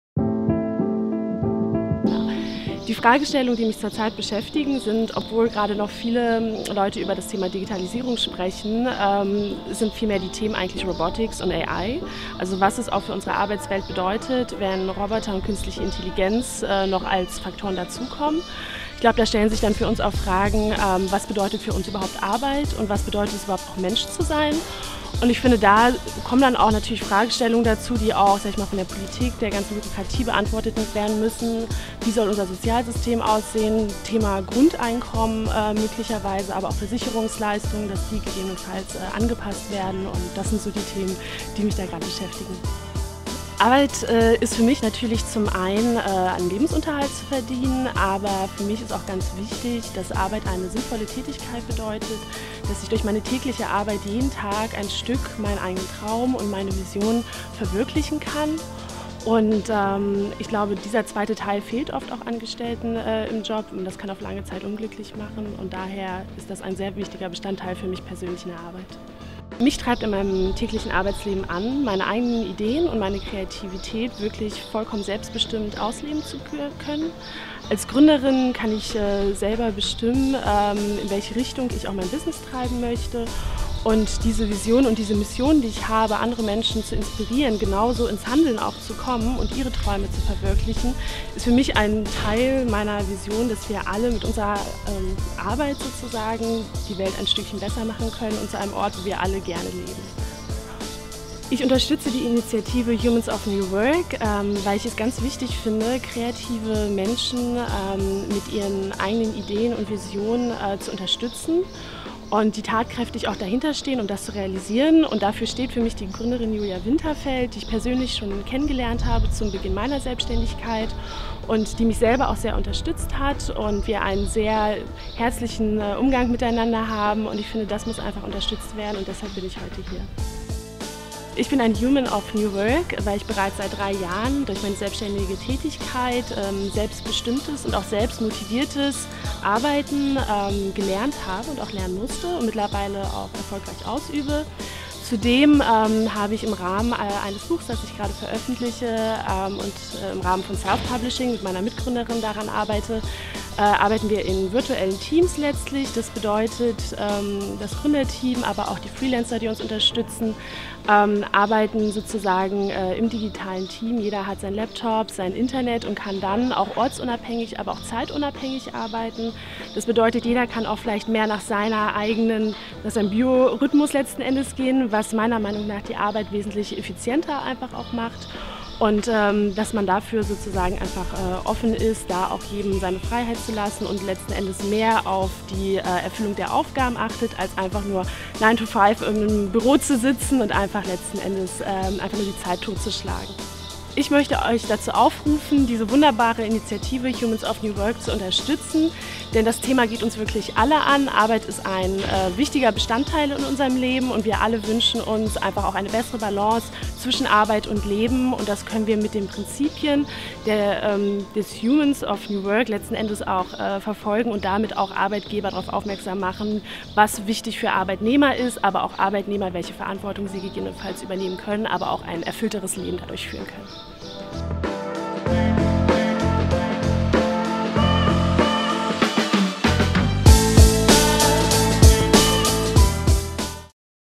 Deine Stimme für Humans Of New Work! Maxi Knust, fempreneur
Maxi Knust ist Gründerin von fempreneur, das Online-Magazin für Female Entrepreneurs.